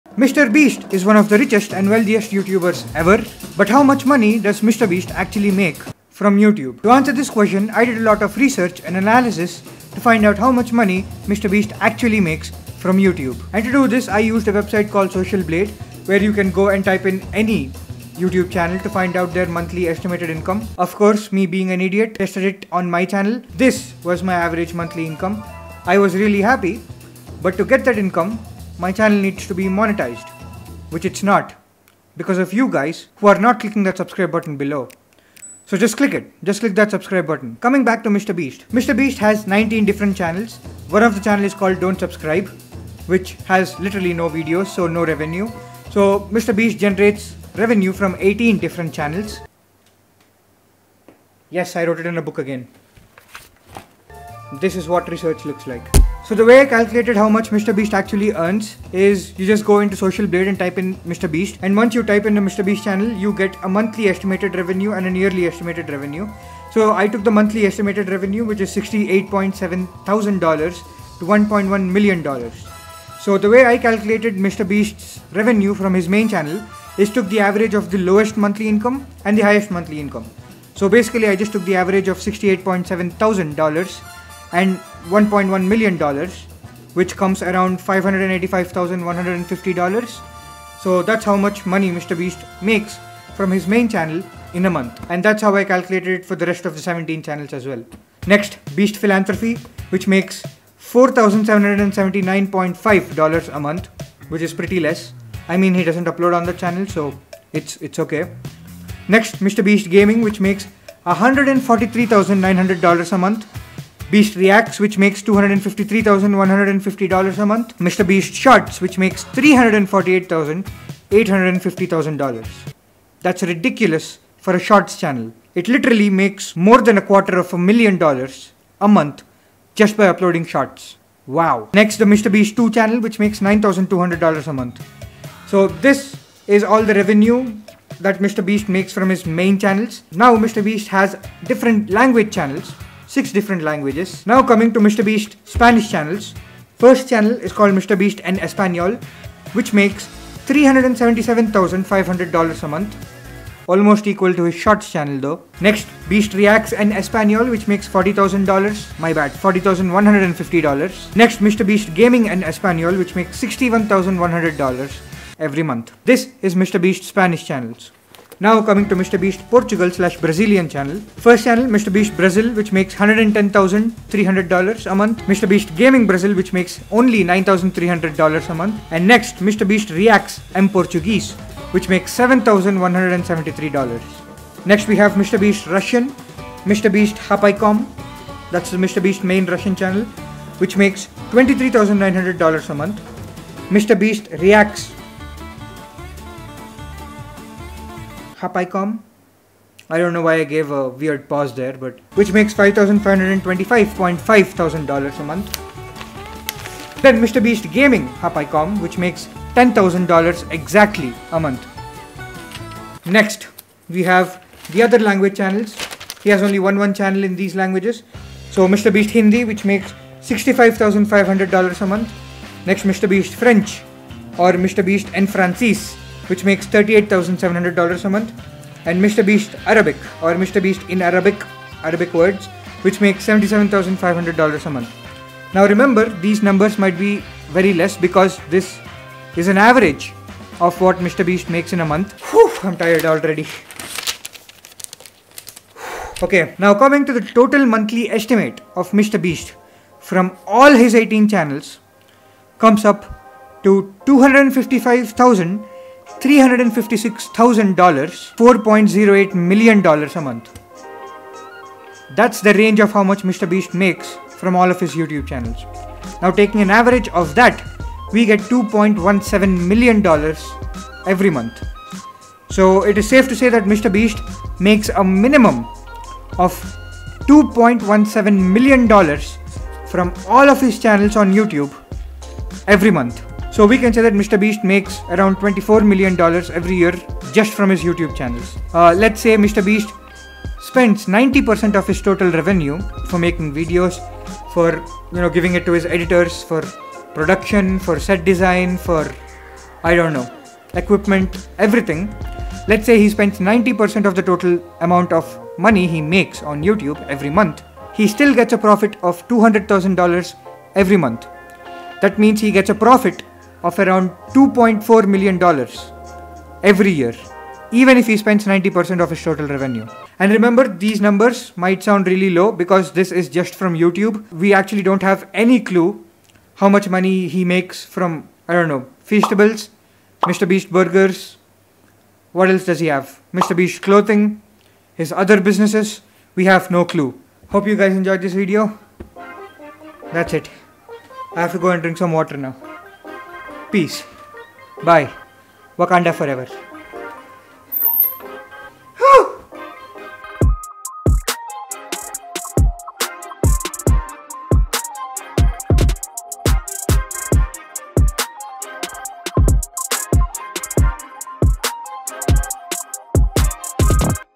How Much Money Does MrBeast Actually Make from YouTube?
MrBeast is one of the richest Youtubers of all time, but how rich is MrBeast?
I did a lot of intense research and analysis and calculated an estimated amount of money that MrBeast actually makes from all of his 19 YouTube channels to answer the question,